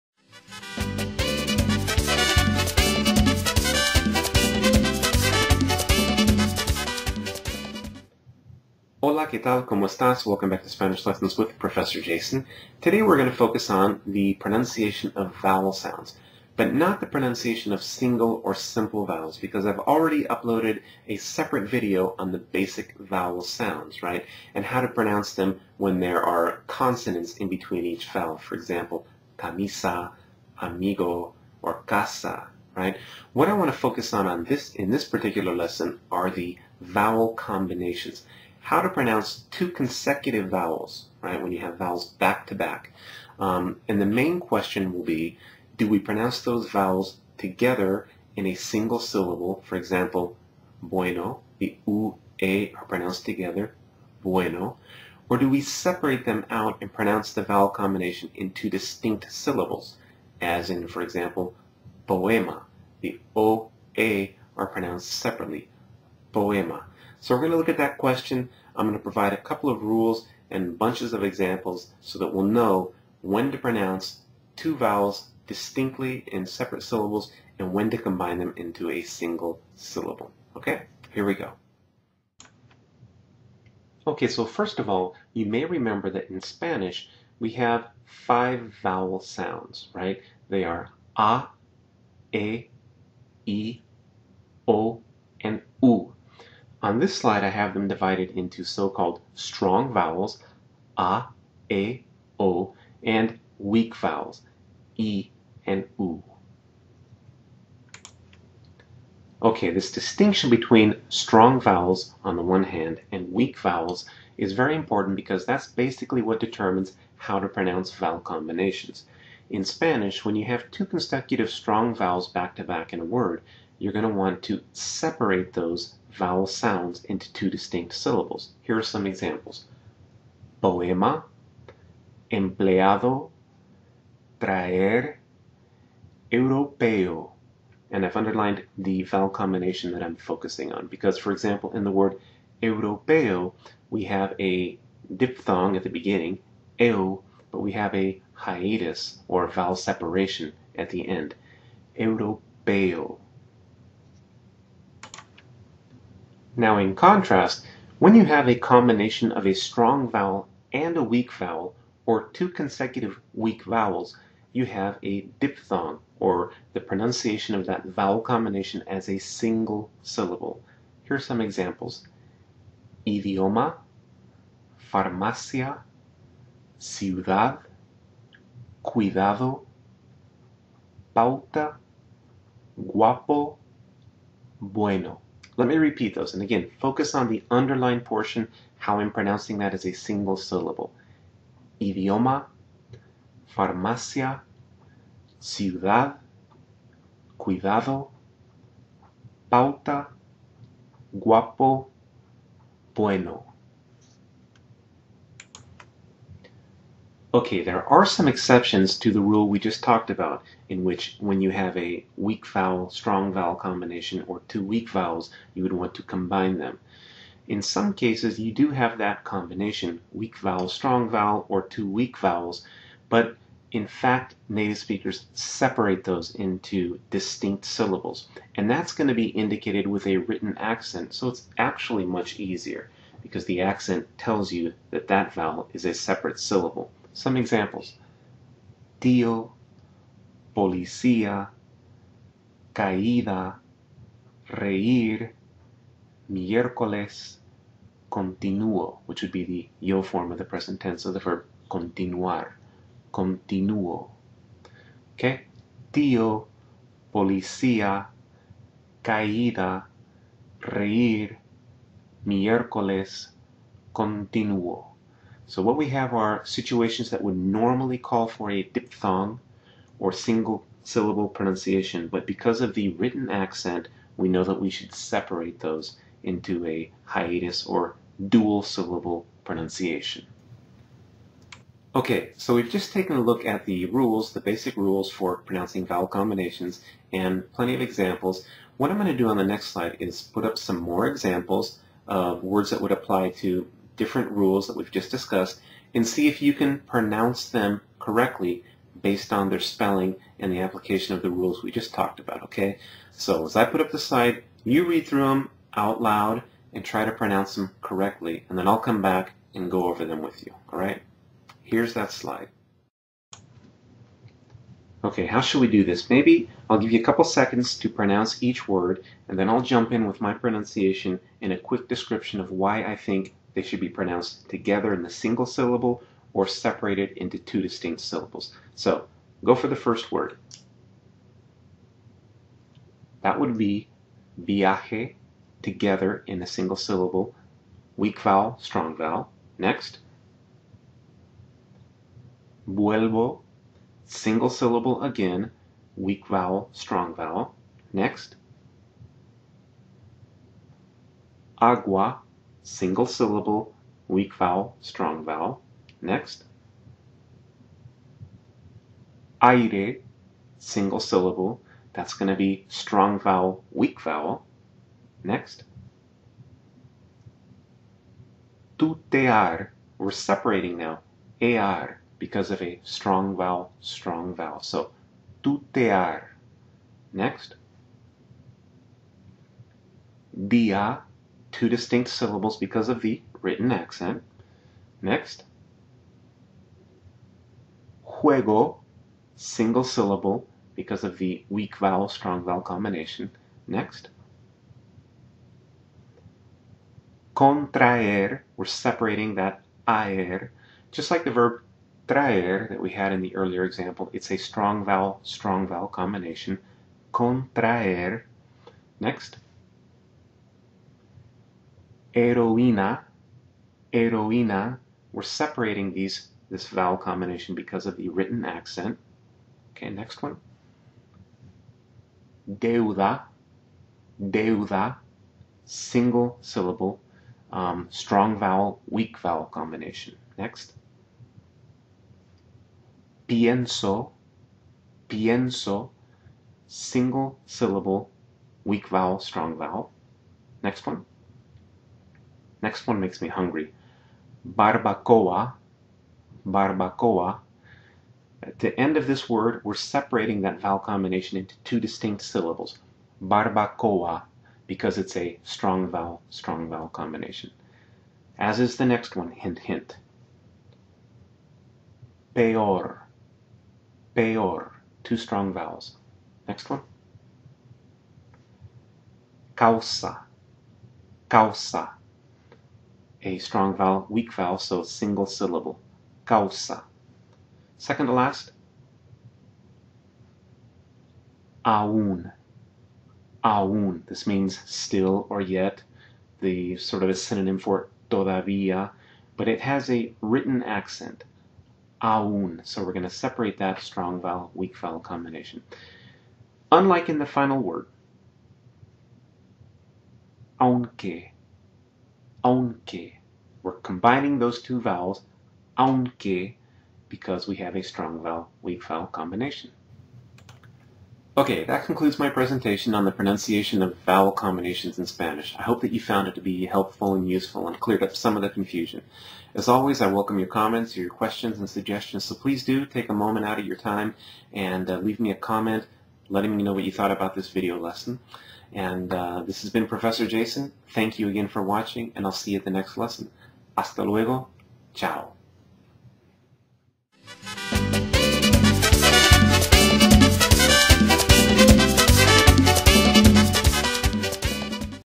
Pronouncing Spanish Vowel Combinations (Diphthongs/Hiatus)
In this lesson I cover the rules for pronouncing vowel combinations, or two consecutive vowels, in Spanish. Basically, the issue is whether to pronounce them together, in a single syllable (diphthong), or as two distinct syllables (hiatus). Here are the rules and some examples...